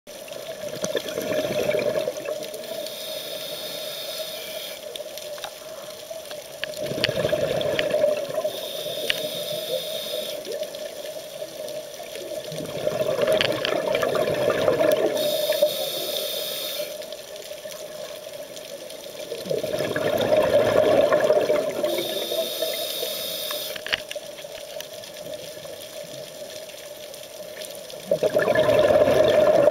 Diving Ni'ihau: Spotted Eagle Ray
Short video of some Spotted Eagle Rays we encountered while diving near the island of Ni'ihau off the coast of Kauai.